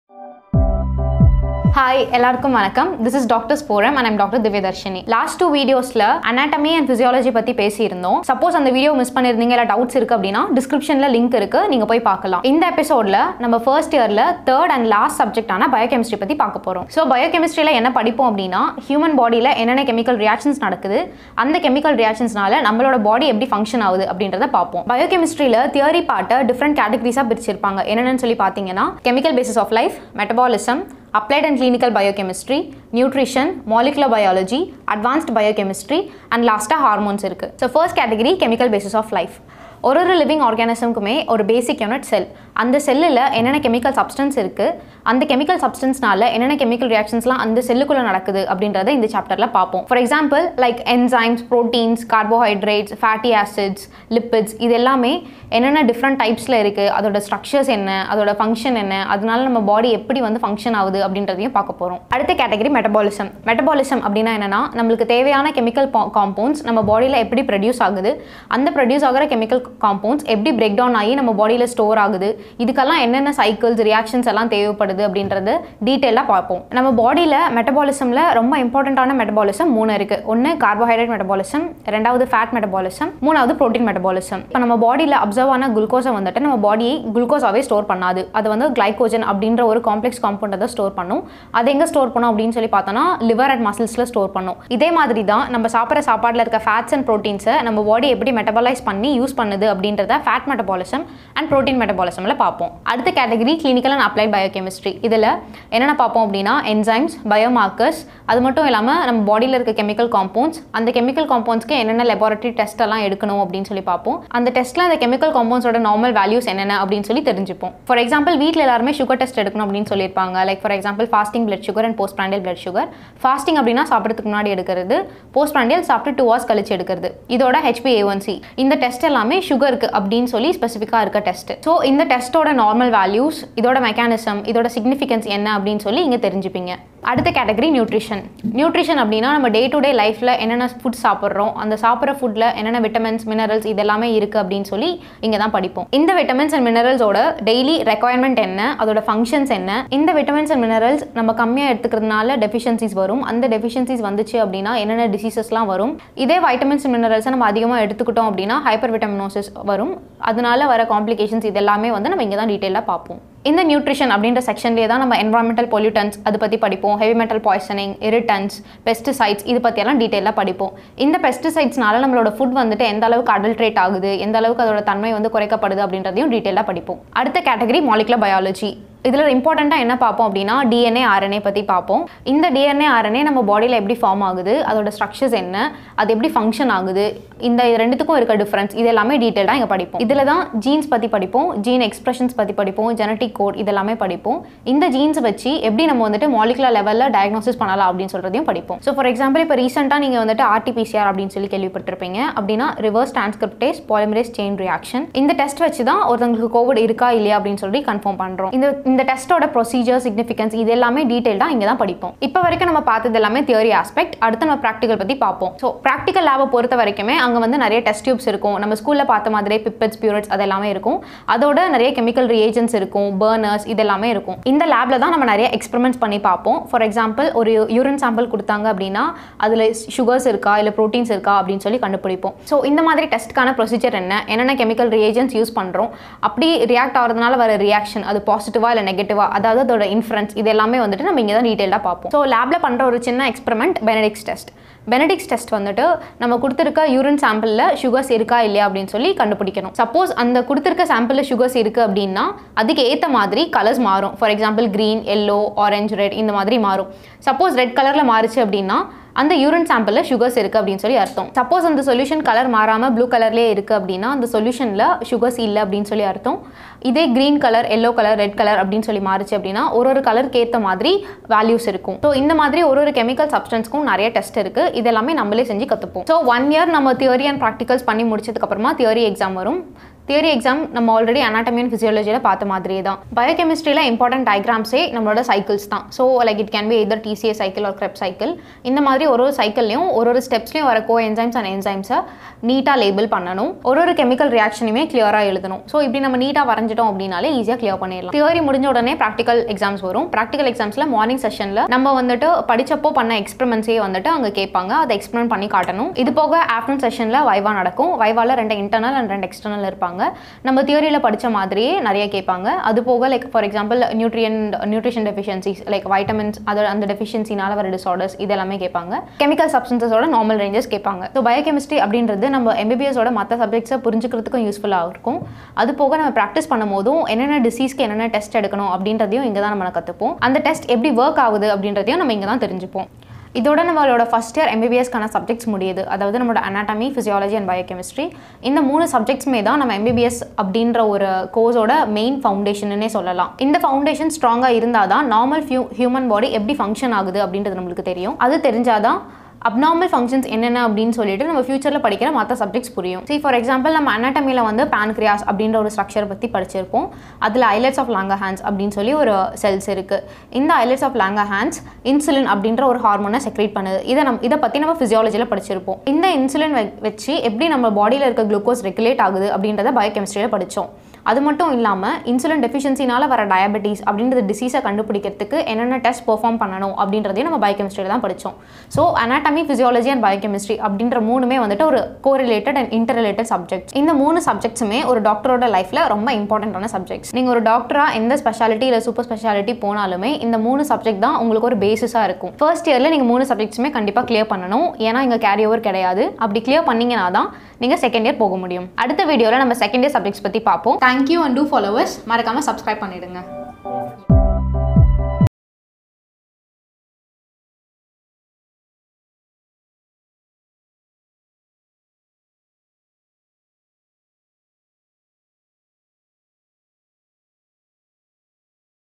MBBS-ல் என்ன படிப்போம் | BIOCHEMISTRY | All about MBBS | Episode-3 | Dr.S.Divyadharshini
Dr.S.Divyadharshini explains about the MBBS First year Subject BIOCHEMISTRY.
This series explains everything about the MBBS course and the subjects to be learnt by Doctors.

KINGWOODS TV